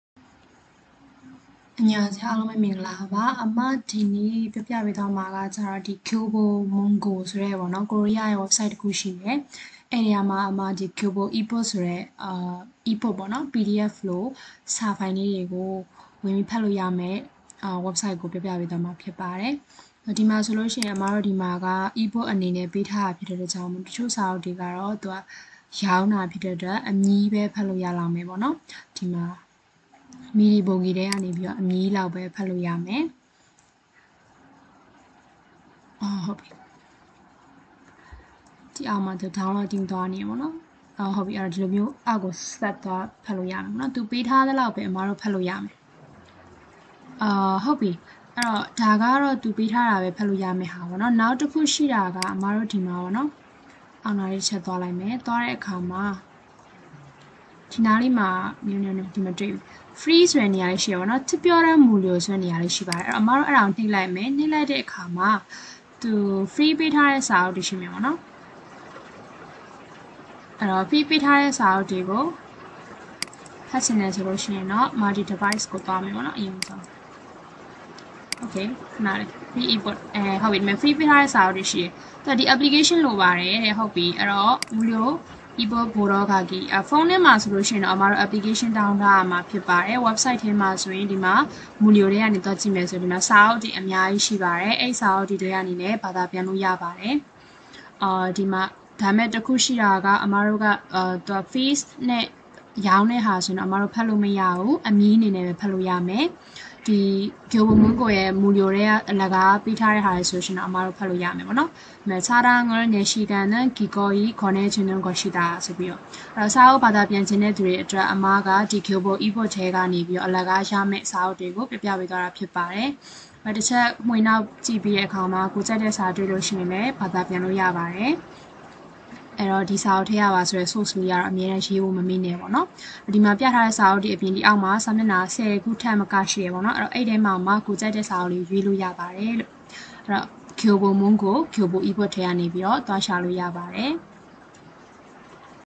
Korean Ebook Free
Kyobo ebook

ကိုရီးယားနဲ့ သက်ဆိုင်သော ebook Free ဖတ်လို့ရနိုင်မယ့် Kyobo ebook

Translator Team members များအတွက်  အရင်က ရိုက်ကူးထားသော Video ကို